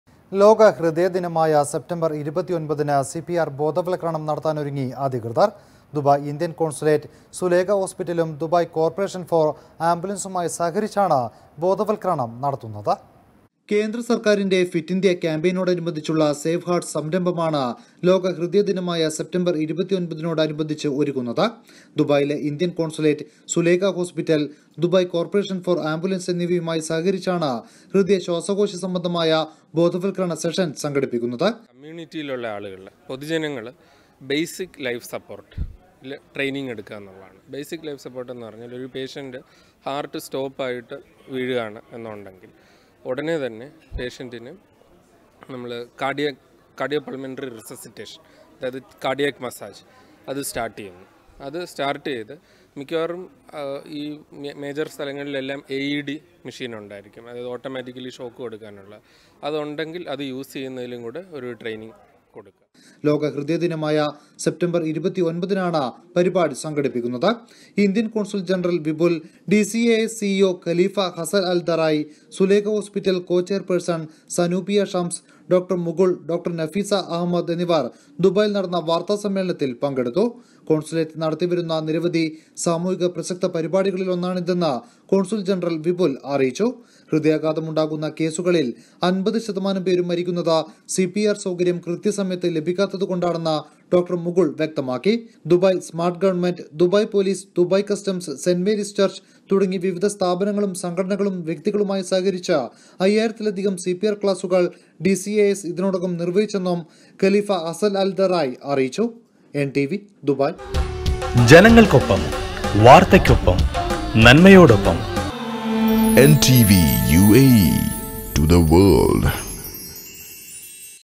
ലോകഹൃദയ ദിനമായ സെപ്റ്റംബര്‍ 29ന് സിപിആര്‍ ബോധവത്കരണം നടത്താന്‍ ഒരുങ്ങി അധികൃതര്‍.
ദുബൈ ഇന്ത്യന്‍ കോസുലേറ്റ്  സുലേഖ ഹോസ്പിറ്റലും, ദുബൈ കോര്‍പറേഷന്‍ ഫോര്‍ ആംബുലന്‍സുമായി സഹകരിച്ചാണ് ബോധവത്കരണം നടത്തുന്നത്.